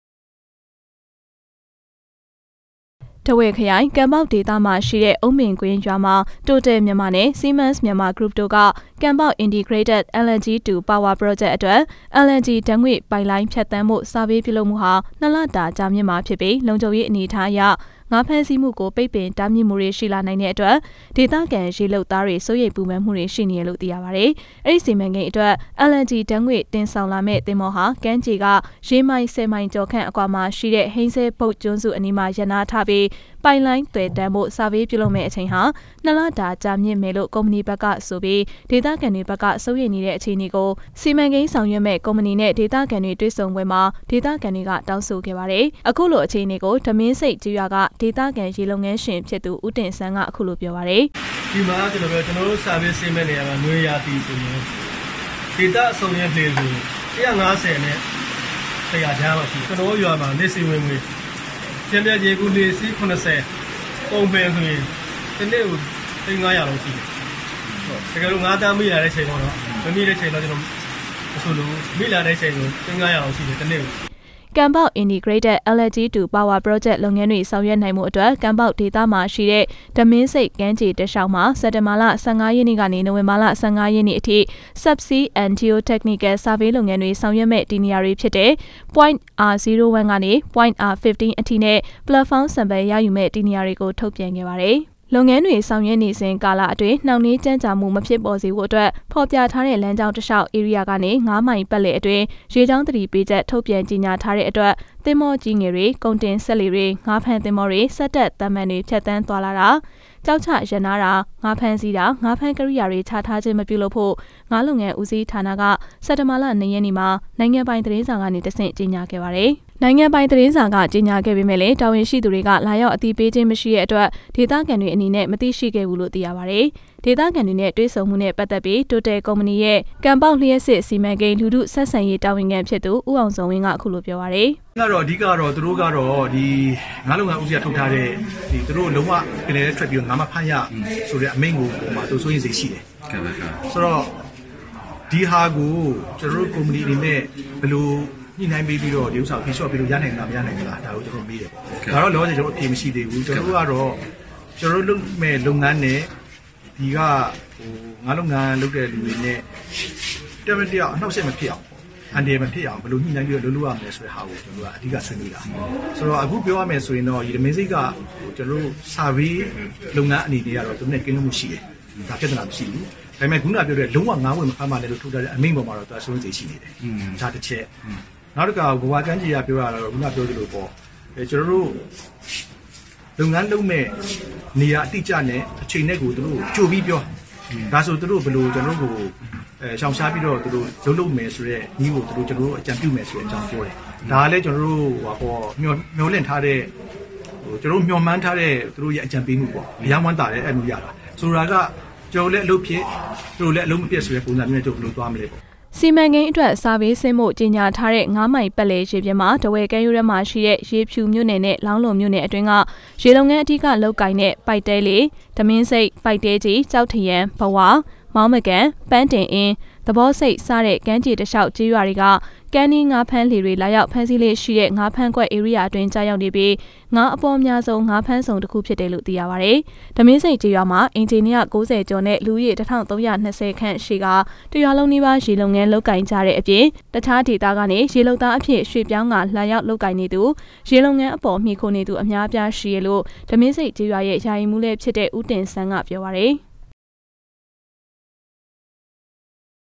ကံေပါက္ေဒသတြင္္ LNG ဓါတ္ေငြ႕ပိုက္လိုင္းျဖတ္သန္းရန္
ကံေပါက္ေဒသတြင္္ LNG ဓါတ္ေငြ႕ပိုက္လိုင္းျဖတ္သန္းရန္ Survey ျပဳလုပ္ ေနစဥ္အတြင္းငါးဖမ္းဆီးမွဳ ပိတ္ပင္တားျမစ္ ခံရမည္ကုိေဒသခံေရလုပ္သားမ်ားစိုးရိမ္ေန
ထားဝယ္ခရိုင္ ကံေပါက္ေဒသရွိအုန္းပင္ကြင္းရြာမွာTotal Myanmar နဲ႔ SIEMENS Myanmar Group တို႔ကKanbauk Integrated LNG to Power Project အတြက္ LNG ဓါတ္ေငြ ့ပိုက္လိုင္းျဖတ္သန္းဖုိ႔ Survey ျပဳလုပ္မွဳဟာ ၂ လတာ ၾကျမင့္မွာျဖစ္ျပီးလံုျခံဳေရးအေနအထားအရ ငါးဖမ္းဆီးမွဳကိုပိတ္ပင္တားျမစ္မွဳမ်ား ရွိလာနိုင္တာေၾကာင့္ ေဒသခံေရလုပ္သားေတြ စိုးရိမ္ပူပန္မွဳေတြ ရွိေနၾကတယ္လုိ႔ သိရပါတယ္။